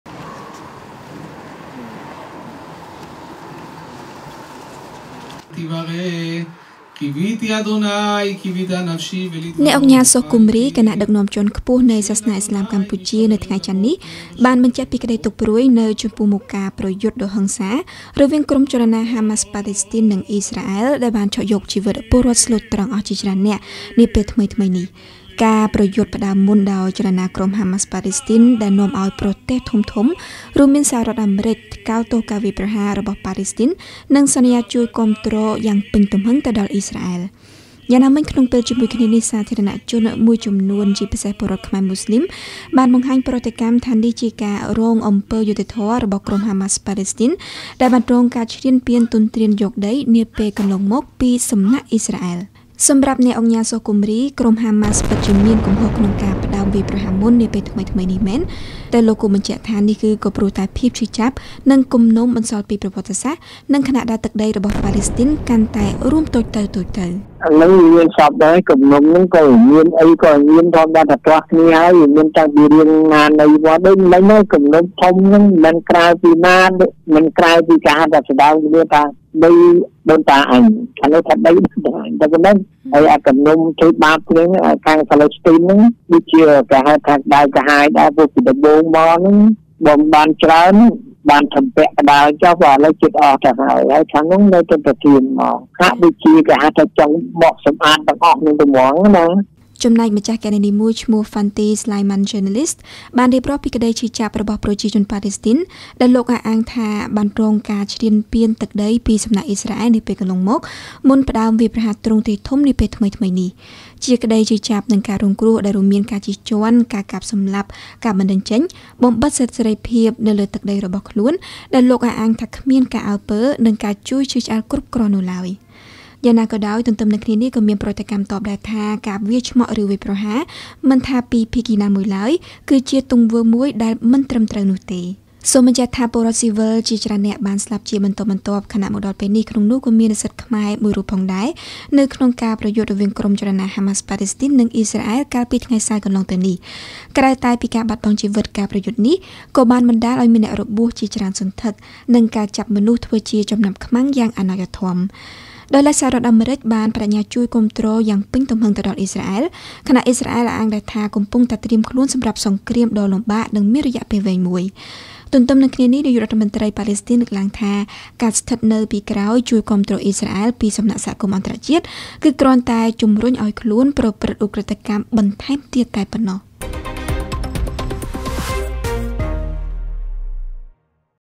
អ្នកឧកញ៉ា​ សុះ កំរី៖​ ហាម៉ាសវាយប្រហារព្រោះតែគំនុំ ខណៈអ៊ីស្រាអែលចង់កម្ចាត់ប៉ាឡេស្ទីនអស់គ្មានសល់
នៅចំពោះមុខសហរដ្ឋអាមេរិកចេញមុខថ្កោលទោសក្រុមហាម៉ាសនិងសន្យាគាំទ្រអ៊ីស្រាអែលយ៉ាងពេញទំហឹង ក្រោយផ្ទុះជម្លោះរវាងភាគីទាំងពីរនាពេលថ្មីៗនេះ បាននាំឲ្យមានការបង្ហាញទស្សនៈម្រូងចម្រាស ជាពិសេសនៅក្នុងចំណោមពលរដ្ឋខ្មែរមូស្លីម អំពីភាពរងអយុត្តិធម៍របស់ក្រុមហាម៉ាសប៉ាឡេស្ទីន។ ក្រៅតែពីបង្ហាញក្ដីទុក្ខព្រួយចំពោះសង្គ្រាមដ៏រន្ធត់នេះ អ្នកឧកញ៉ា​ សុះ កំរី គណៈដឹកនាំជាន់ខ្ពស់នៃសាលាឥស្លាមកម្ពុជា យល់ឃើញថា ការផ្ដើមវាយប្រហារមុនពីសំណាក់ក្រុមហាម៉ាស គឺបណ្ដាលមកពីគំនុំនិងភាពឈឺចាប់បន្សល់ពីប្រវត្តិសាស្រ្ត។